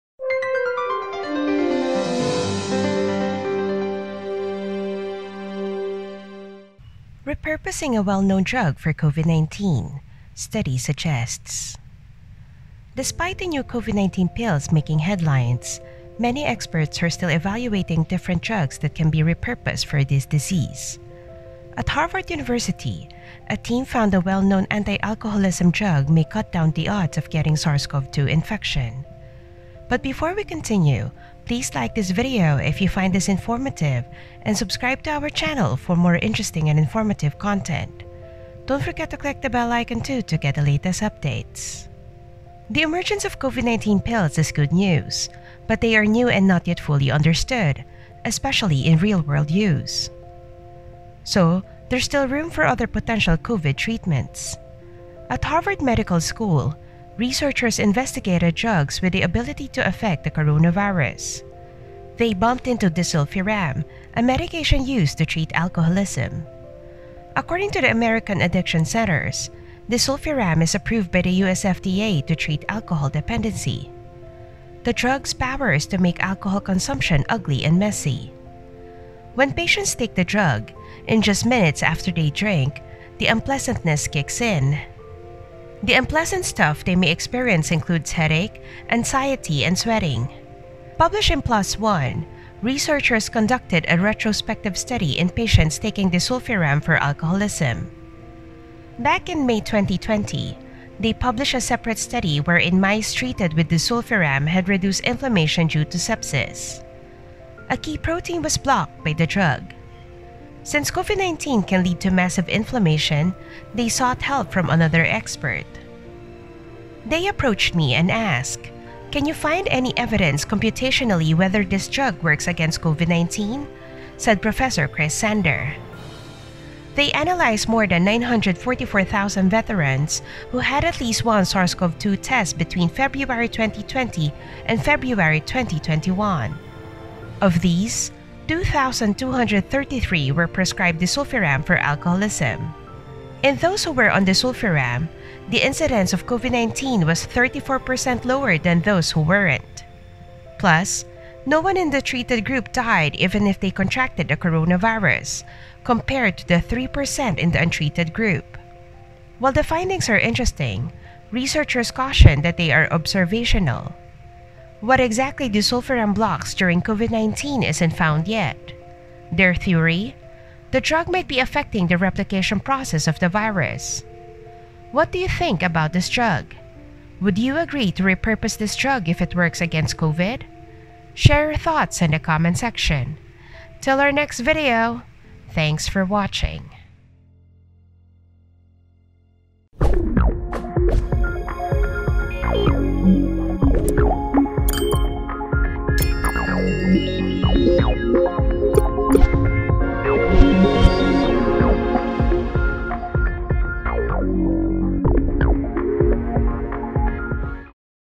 Repurposing a Well-Known Drug for COVID-19, Study Suggests
New research shows the possible potential of a familiar drug against COVID.